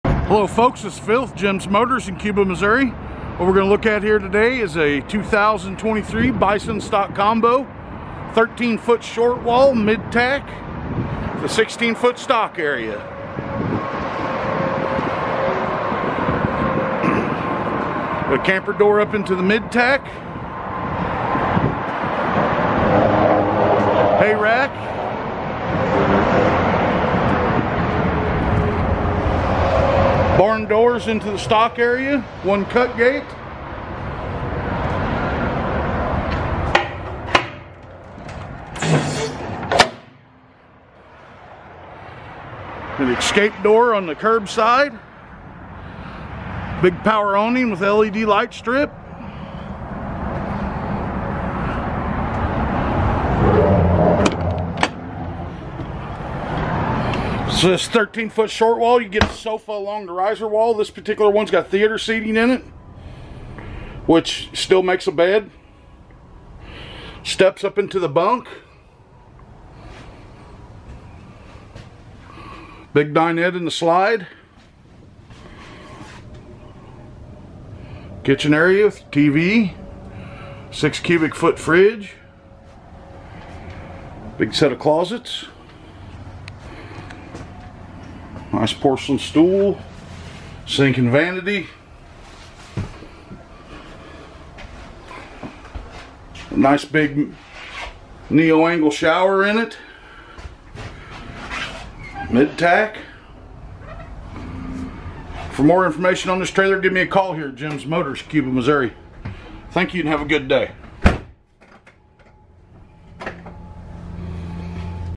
BISON STOCK COMBO LQ FOR SALE
215/75R17.5LRH Cooper Radial Tires W/ Black Alum Wheels
3-Way Switch for Stock Area
30 Amp Service W/ Detachable 30 Amp Power Cord
50/50 Rear Load Doors
8000# Axles
Bridle Hooks in Mid-Tack Area
Bunk Privacy Curtain
Exterior Water Spigot (N/A 6′ Layout)
Furnace, 18,000 BTU
Glass Shower Door
LED Exterior Lights
LED Lighting Throughout
LP Tanks (2) W/ Dual Regulator
Manual Jack – 2 Speed
Microwave
Porcelain Toilet
Radius Shower W/Dual Control Handles
Rear Load Light (1)
Recessed Cook Top W/ Range Hood
Refrigerator
Residential Faucets
Rubber Matting in Mid-Tack & Stock Area Floor
Saddle Tree in Mid-Tack Area
Shirt Closet(S) W/Mirrored Inserts in Bunk Area
Sink Covers
Skylight In Shower
Step (at Entrance Door)
Spare Tire & Rim
Stainless Steel Kitchen Sink
TV/Radio Antenna
Two Batteries, Grp 24 W/ Shut-Off
Upgraded Countertop Throughout
Upgraded Floor Covering Throughout
Upgraded Graphics Package
Vinyl Valances W/Roller Shades
Water Heater Gas DSI (6 Gal)